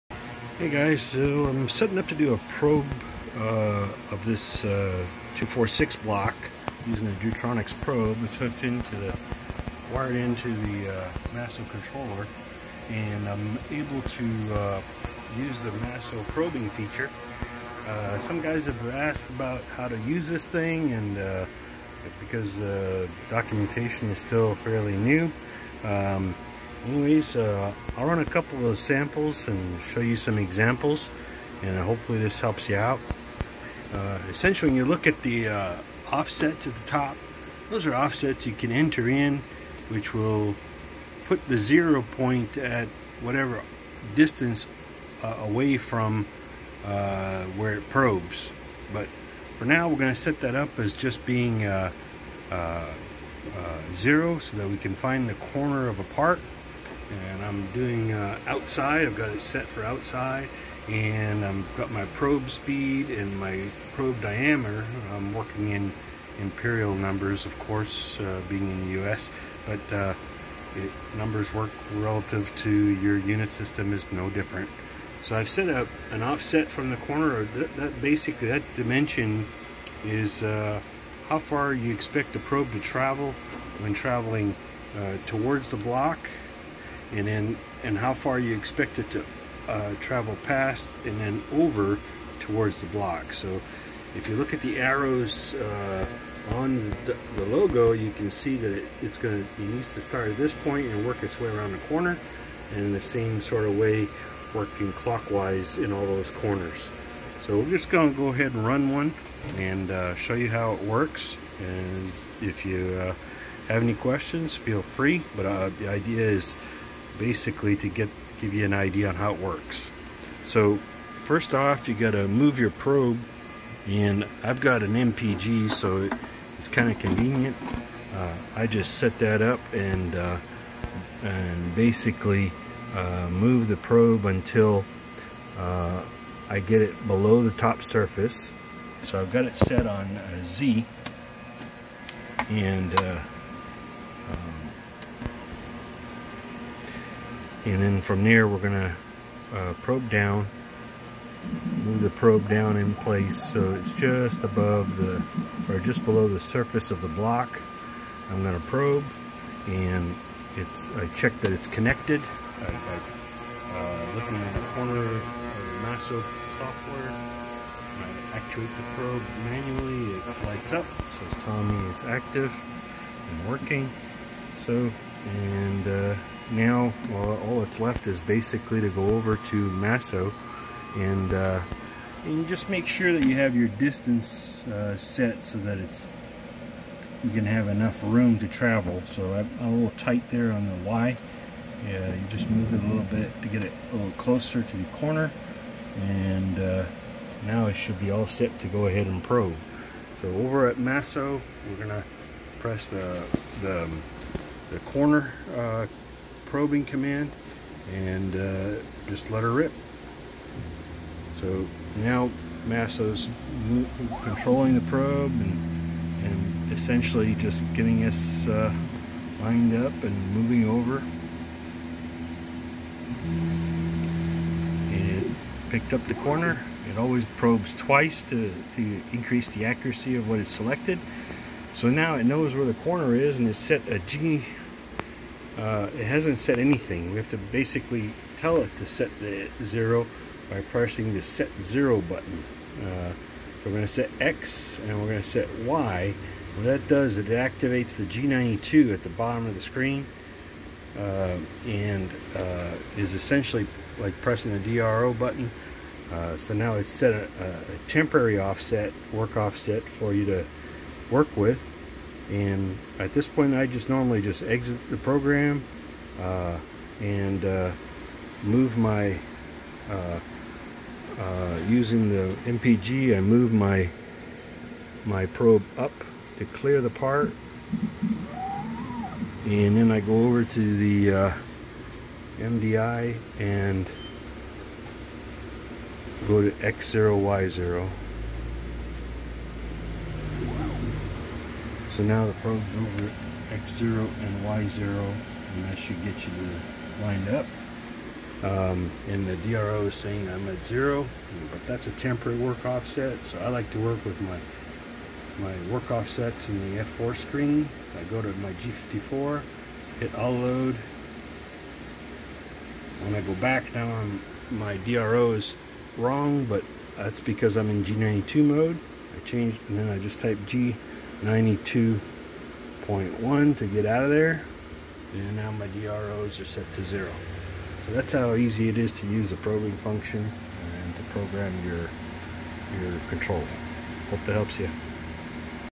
Using the Masso CNC Probing Menu
Just a quick video showing how to use the Masso controllers built in probing menu.